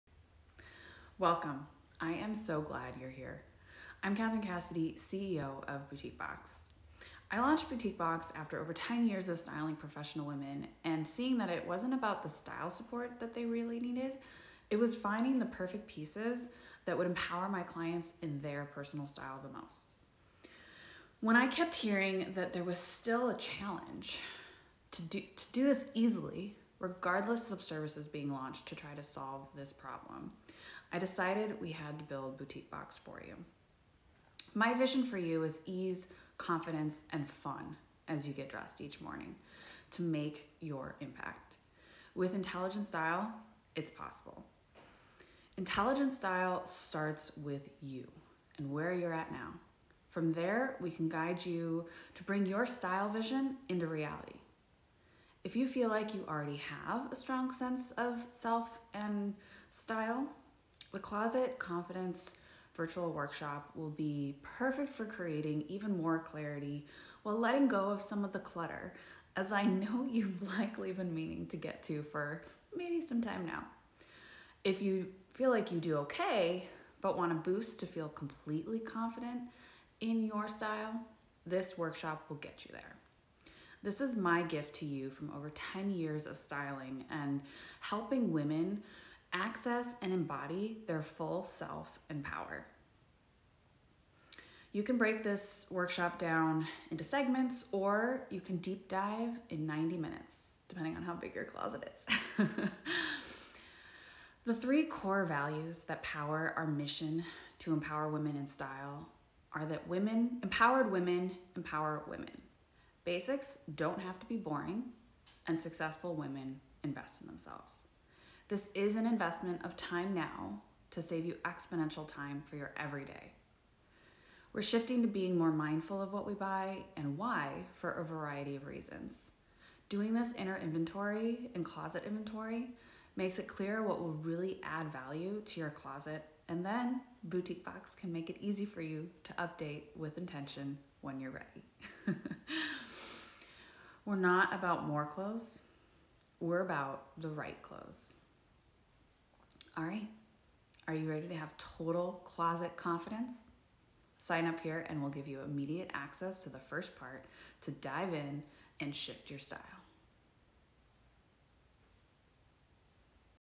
Boutique Box CEO Catherine Cassidy: Closet Confidence Invitation
At Boutique Box we're driven by the truth that clothes are a catalyst for confidence and it's not about more clothes, but the right clothes. Confidence comes from Clarity, which our free Closet Confidence virtual workshop will give you with EASE. This is a simple 3-part system that gives you the tools you need for total confidence in defining YOUR style.